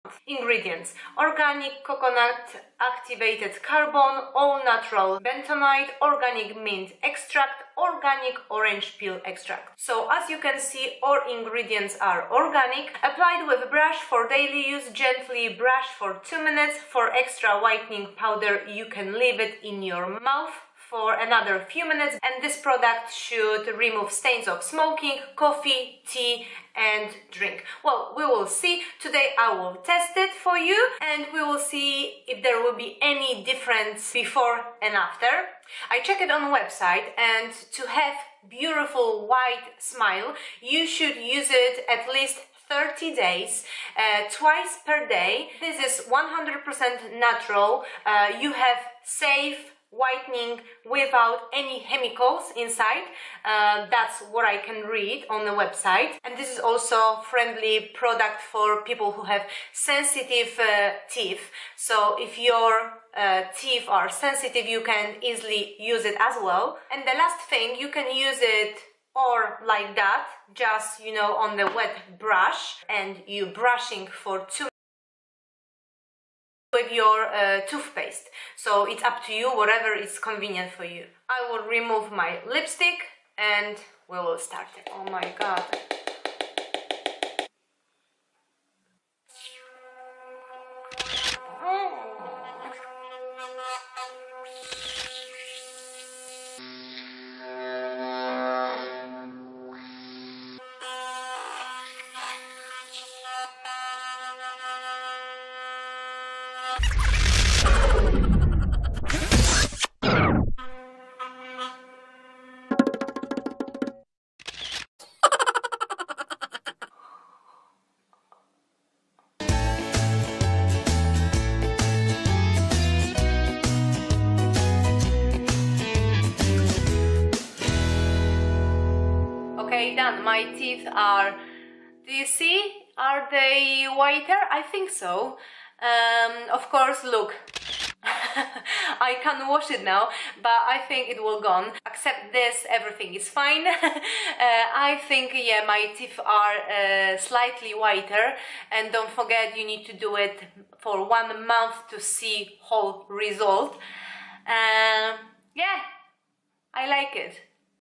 Teeth Whitening Powder
Brand: AZDEN

Description :
Black powder for teeth whitening is a modern alternative to the "ancient" method of cleaning teeth with soda (thanks to small grains, you can get rid of plaque on the surface of the teeth and whiten them a little). In terms of bleaching and cleaning, the powder is no less effective (coconut charcoal does an excellent job of its role), and it tastes much better.
For people who want to whiten their teeth in a short time at home, with high quality, without unnecessary effort.

The unique formula helps to prevent the appearance of stains on the teeth as from coffee, nicotine plaque, as well as the whitening effect, freshens breath.

Advantages:

Absolutely natural remedy;
With a whitening and polishing effect;
Fun to watch yourself in the mirror.

Application:
1. Wet a toothbrush and dip it in powder, the powder has a fresh mint flavor, contains a whitening formula, is a little big.
2. Brush your teeth gently for two minutes, 360 ° whitening.
3. Rinse again with water to remove residue by deep cleaning.
4. Enjoy white teeth, bright smile!
5. Brush your teeth with a whitening product twice a day for a few minutes. It is recommended to use the whitening product every day for several weeks to ensure the best whitening effect.